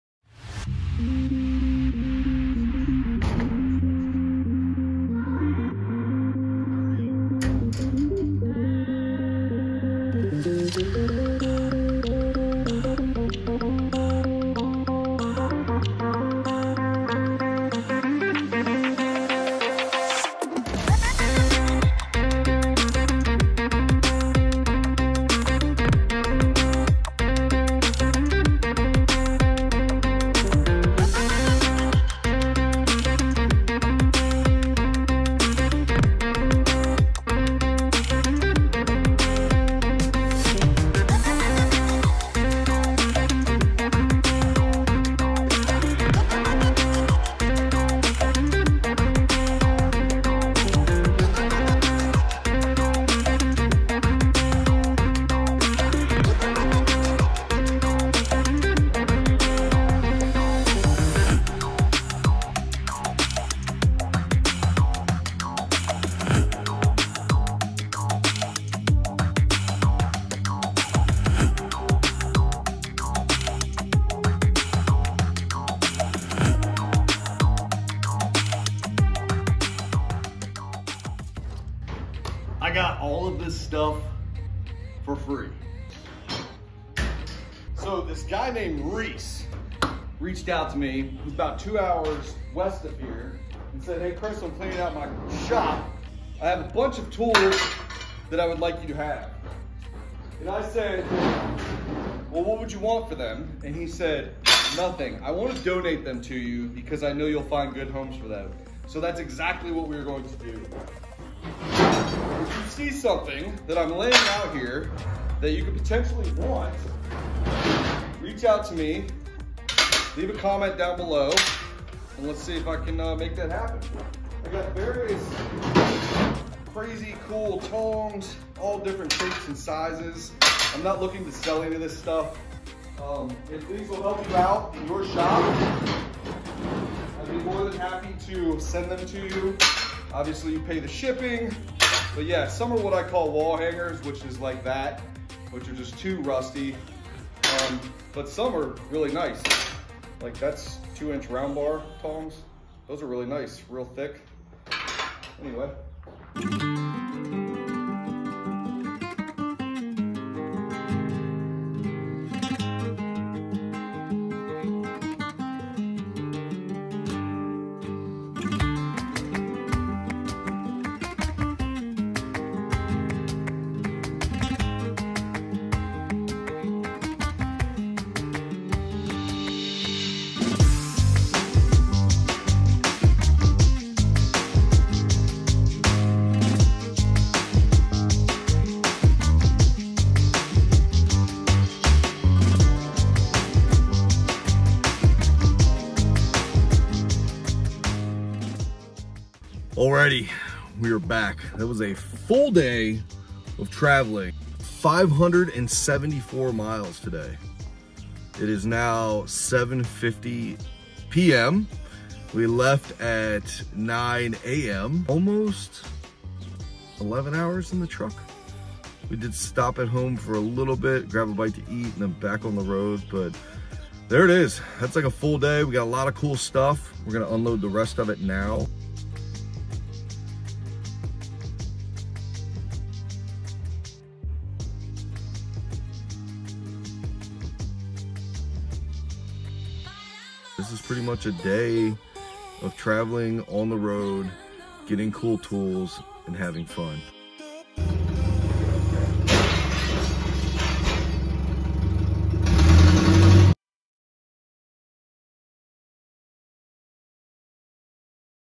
I’m giving away all of these tools!
A day in the life of nonsense and driving!

Reach out if these tools can help you in anyway!

Mt_phillip_metal_works